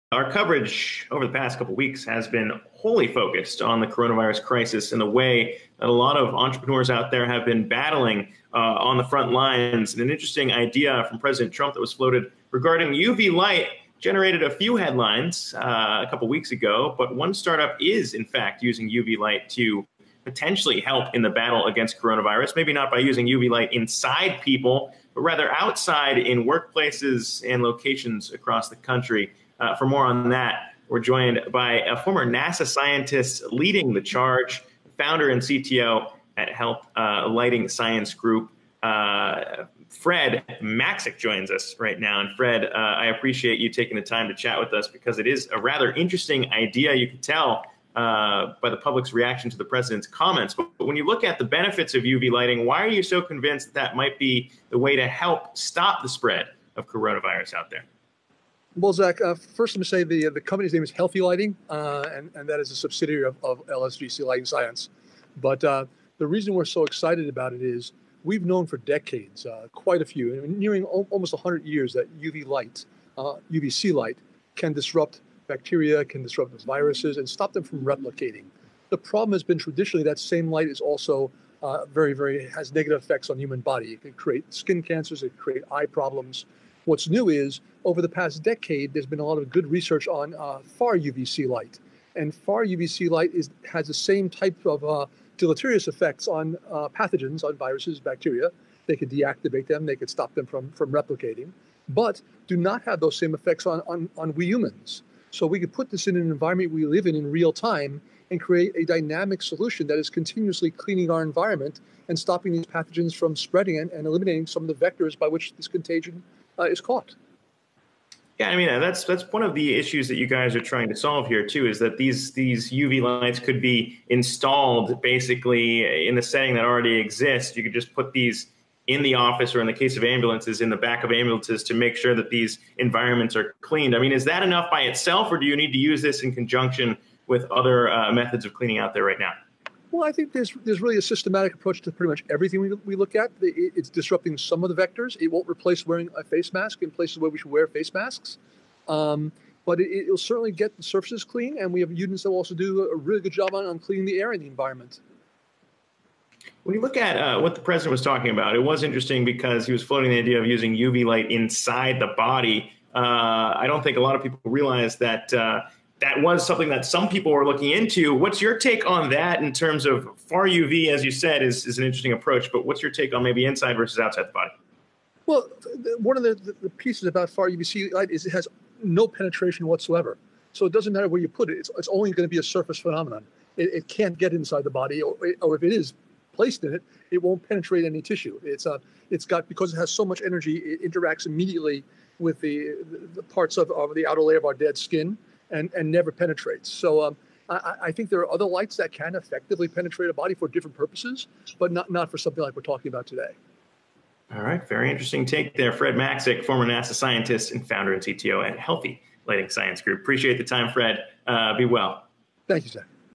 Healthe's Cleanse Far-UVC Light Could Be 'Game Changer' in Coronavirus Fight
Fred Maxik, Former NASA scientist & Founder and Chief Scientific Officer at Healthe Inc., joins Zack Guzman to discuss how UV light may help combat the spread of the coronavirus.

Source: YAHOO! Finance, May 5, 2020